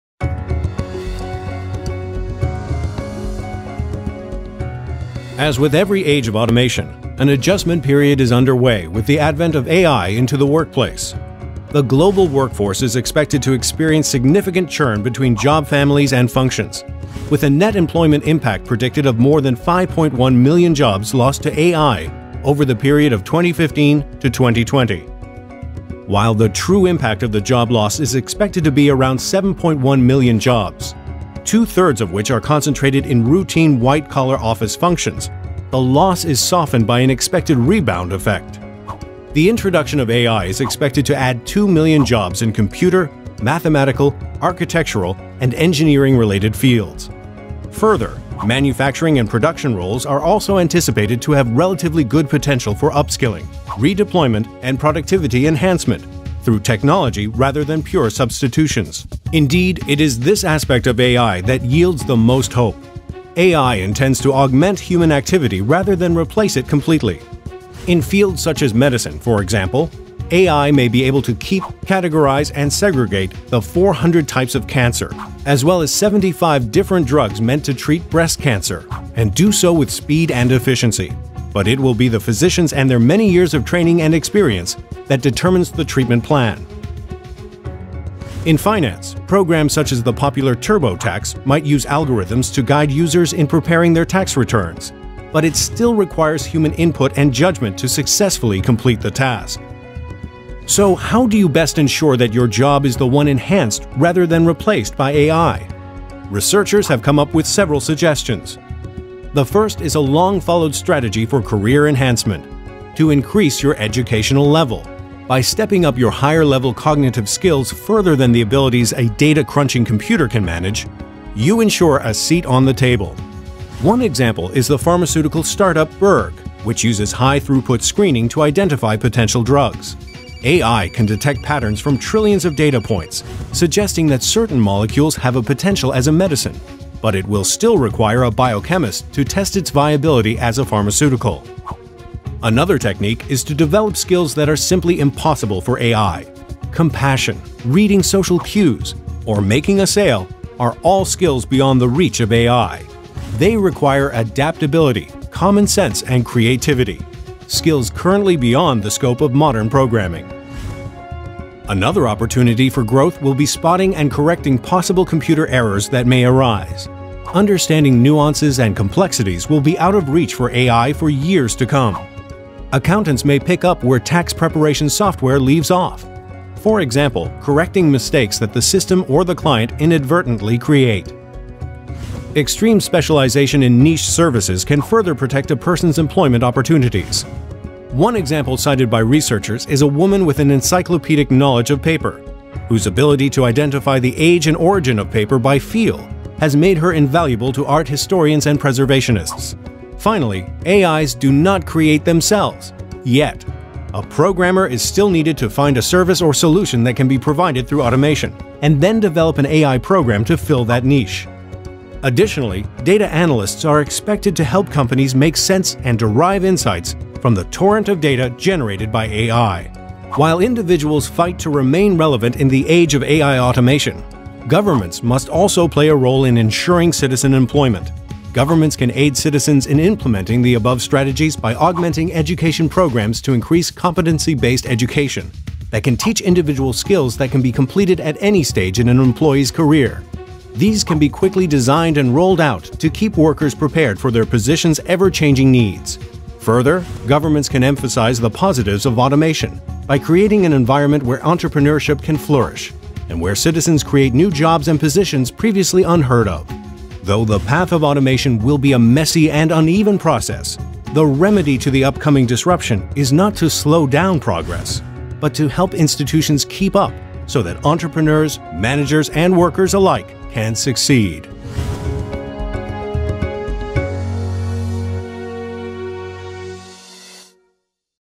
AI at the workplace: Opportunities and Challenges
Description:
AI has entered the workforce, and many are afraid of the massive job loss it will cause. This video explains the jobs and industries that will be affected the most by AI, and those who will benefit from it, the new opportunities it will create, in addition to governments' role in ensuring citizens' employment.

Sources:
Artificial Intelligence: Disrupting the future of work